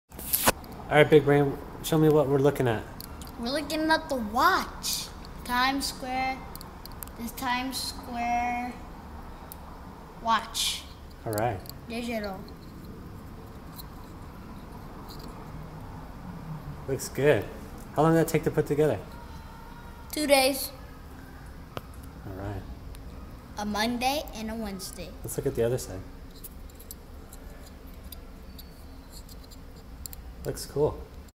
Big-Brained Superhero vs. Timesquare Watch from Adafruit
This 3rd-grade Big-Brained Superhero built and earned his watch by exercising his Persistence superpower. Now for the programming!